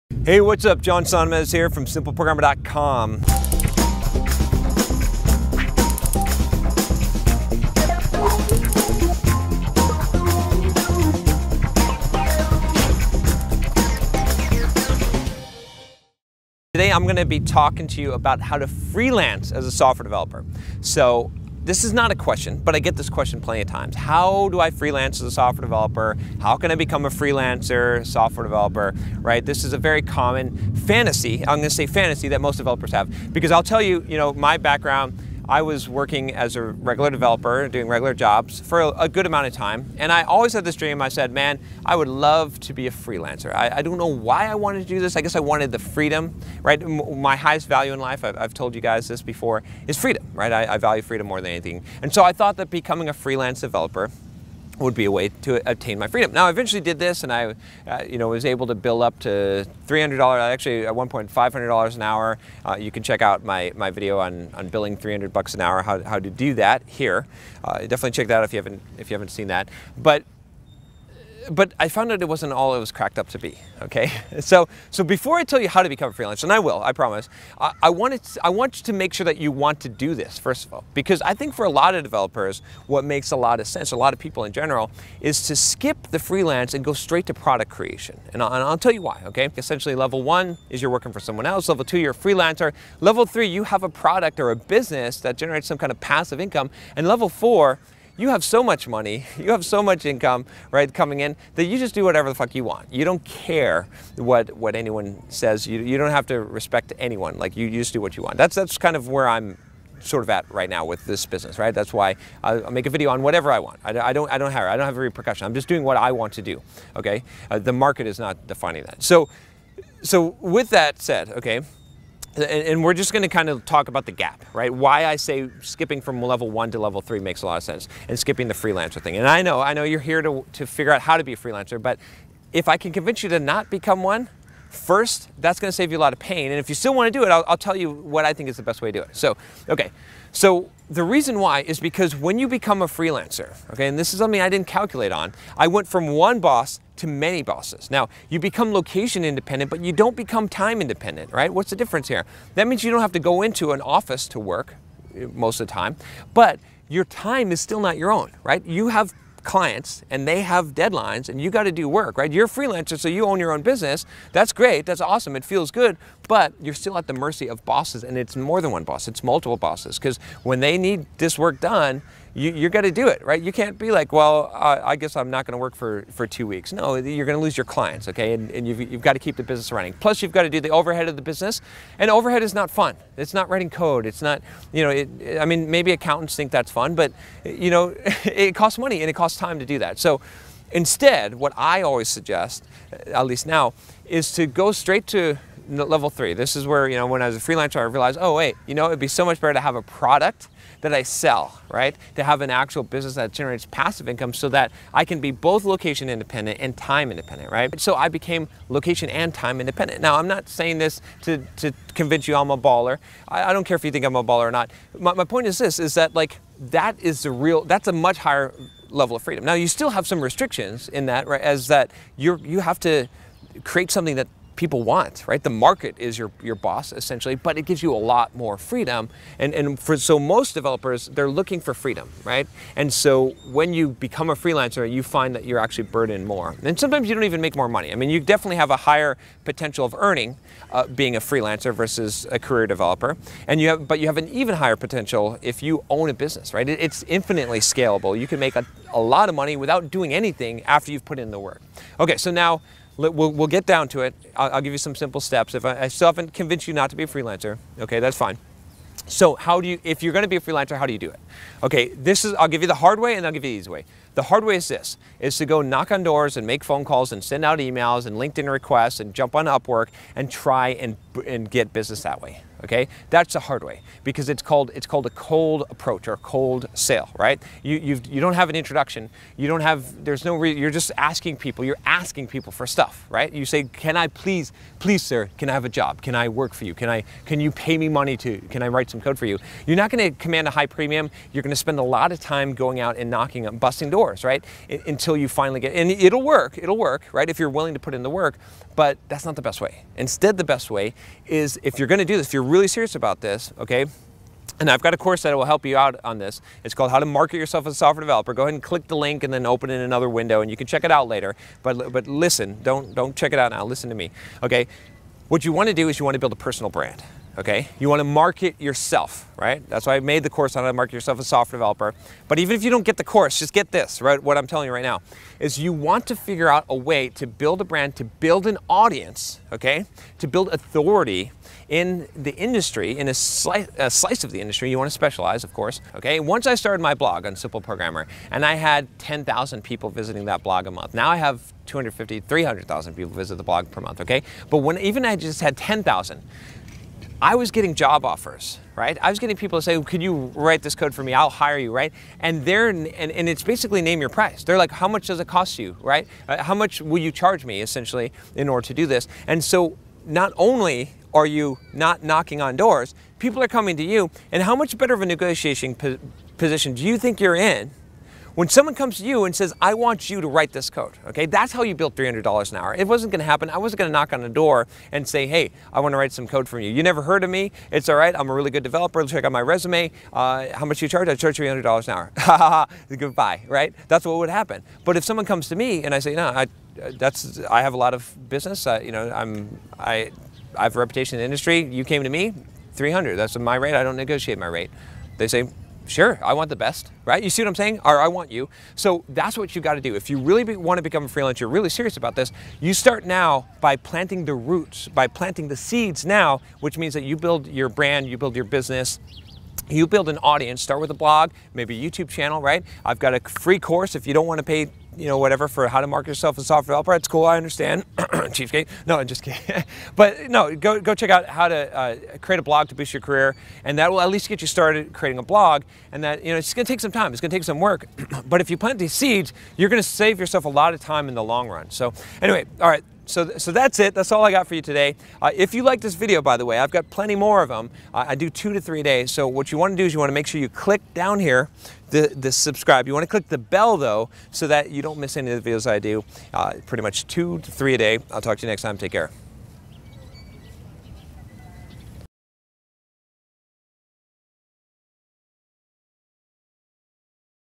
How To Become A Freelance Programmer
Simple Programmer

When I was working a regular 9-to-5 job, I’d often fantasize about starting my own business––working for myself.

I’d think about how nice it would be to be a freelancer and not have a boss. I’d daydream about traveling the world, working whatever hours I pleased, and making hoards of money from lucrative contracts.

Then I’d snap to reality as I realized I had no idea how to go about making something like this happen. I mean, how do you actually become a freelancer and work for yourself?

This is the dream of most software developers. Most of them fantasize about working for themselves, just to start freelancing and discover that things do not work 100% like that.

The first question you should ask yourself is if you are sure whether or not you really want to embark upon this path.

Don’t get me wrong; I want you to become an entrepreneur of sorts.

I want you to eventually stop working for “the man” and find your path to freedom.

But freedom is not for everyone.

The reality is that it is going to take a large amount of work to even get to the point where the dream is a reality.